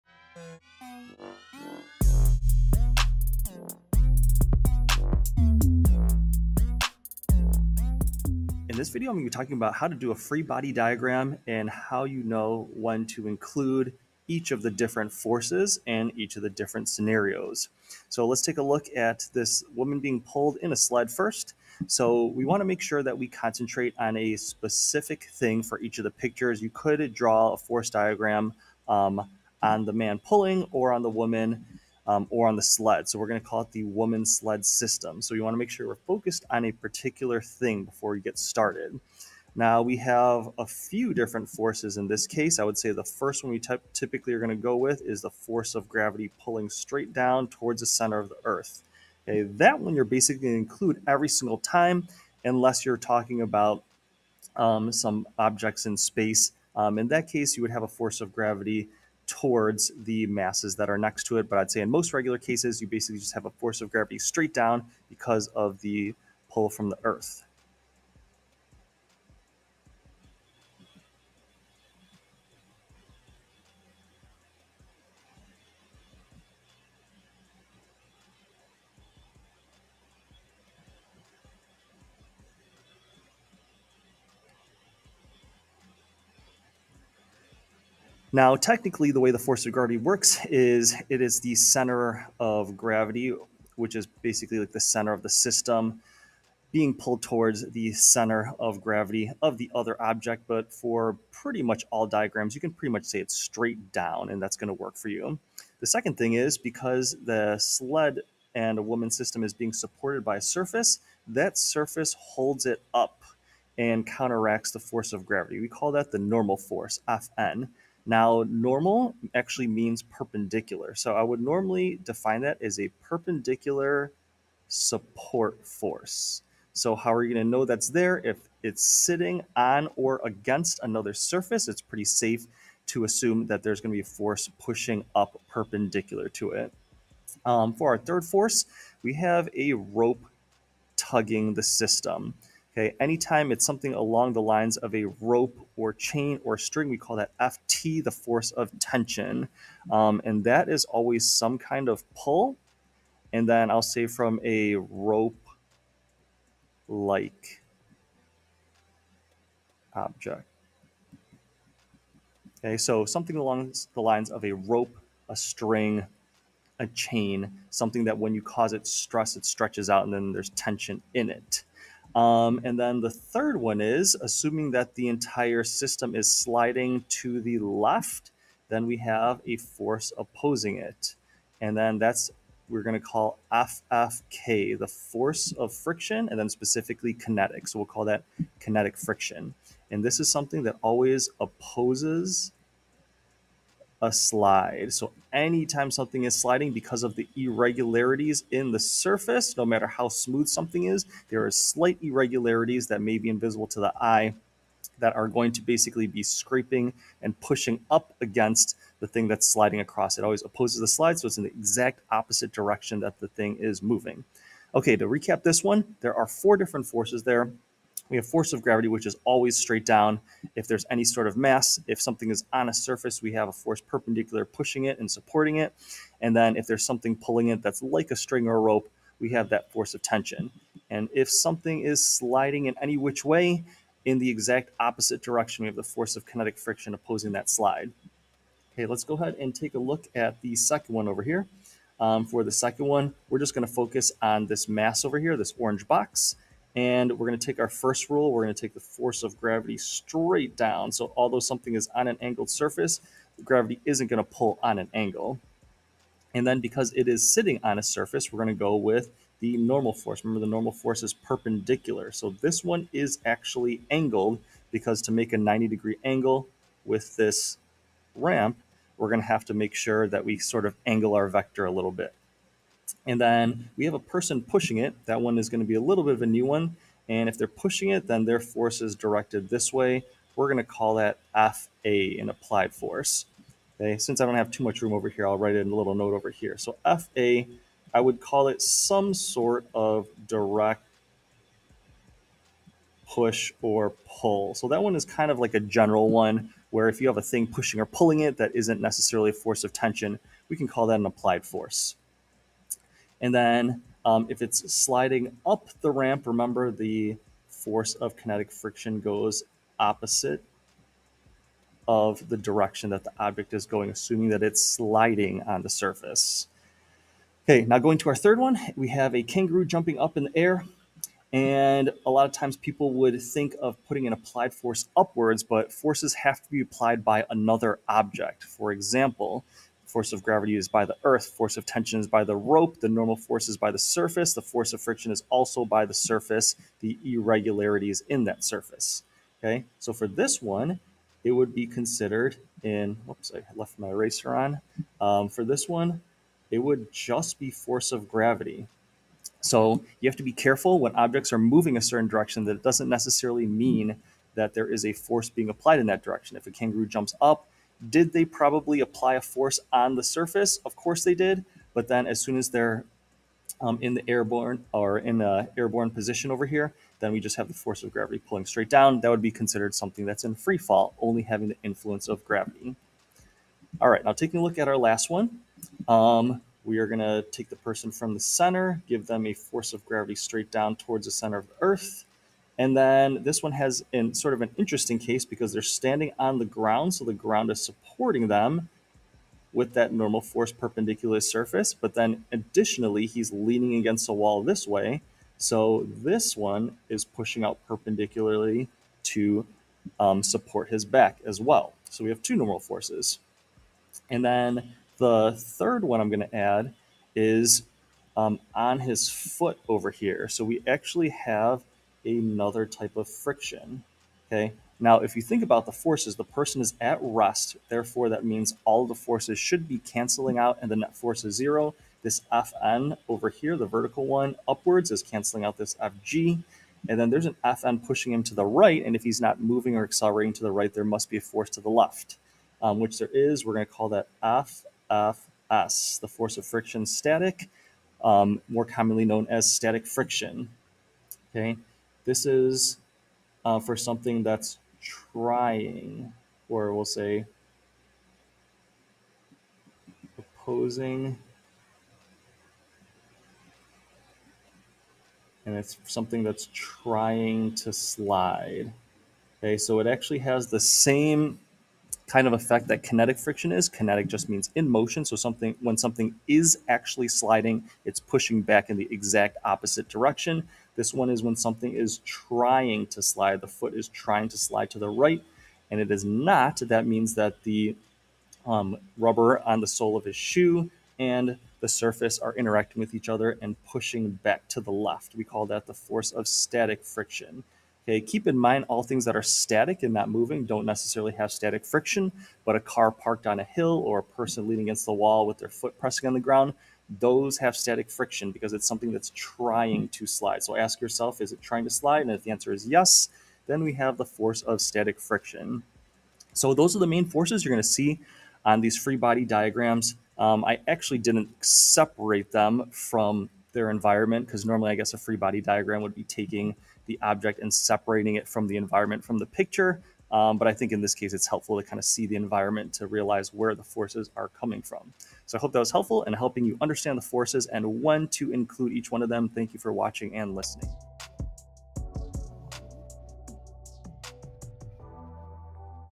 Free-Body Diagrams - Know When to Include Each Force (Fg, Ft, Fa,Fn, Ff)
In this video, scenarios are analyzed to show when to include a force and which directions the vector points as well.  4 examples are worked out to show the thought process how to accurately complete a free-body diagram.